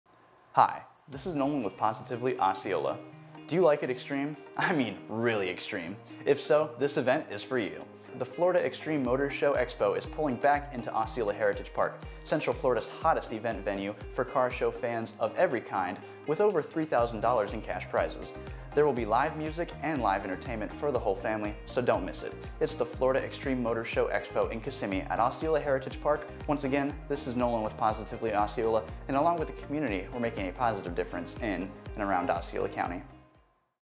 Florida Extreme Motor Show Expo
On Sunday, October 16th at 11am the Florida Extreme Motor Show Expo is pulling back into Osceola Heritage Park, Central Florida’s hottest event venues for Car Show Fans of every kind with over $3,000 in cash prizes. There will be Live music and LIVE entertainment for the whole family!